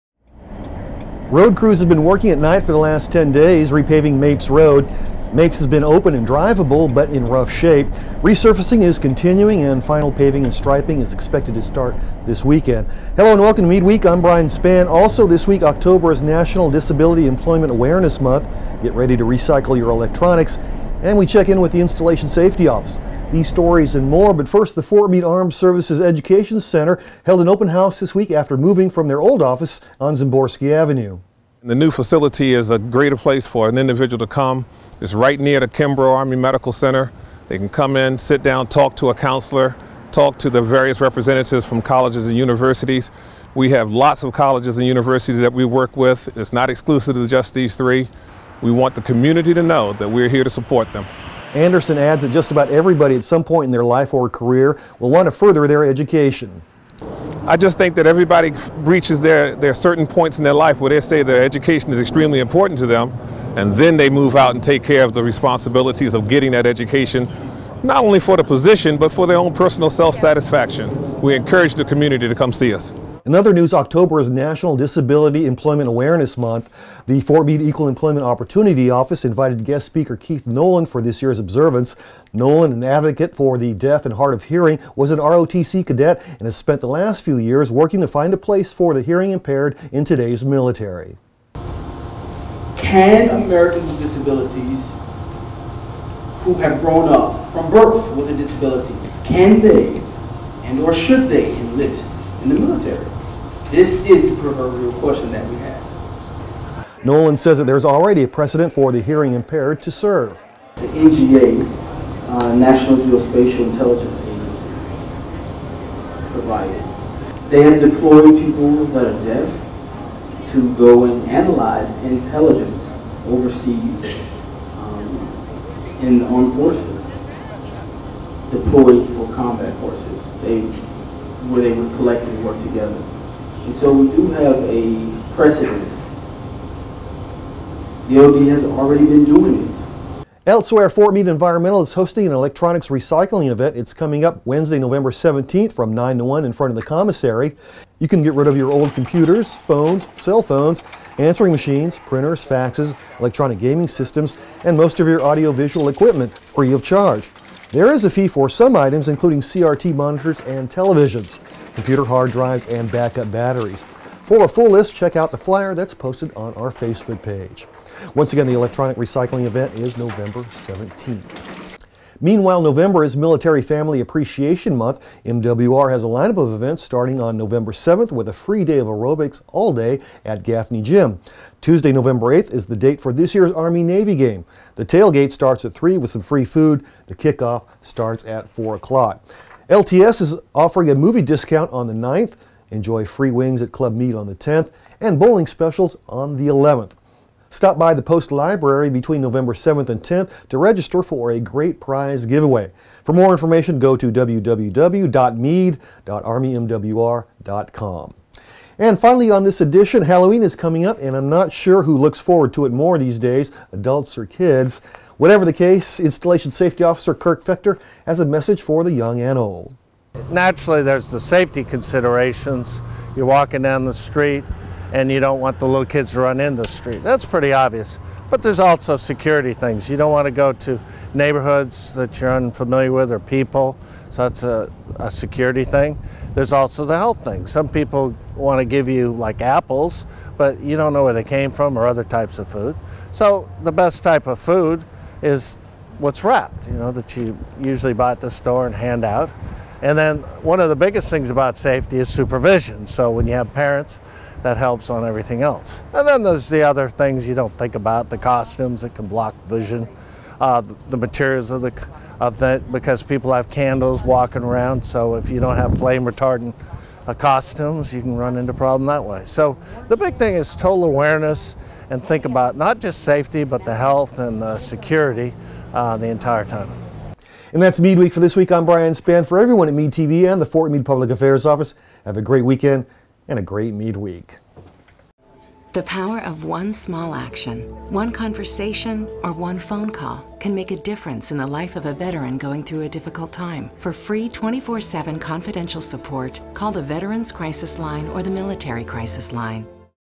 Meade Week for Oct. 28, 2016
Meade Week's a day early this week!  On this edition:  Mapes roadwork continues, October is Disability Employment Awareness Month, get ready to recycle your electronics, and we check in with the Installation Safety Office.  These stories and more on Meade Week!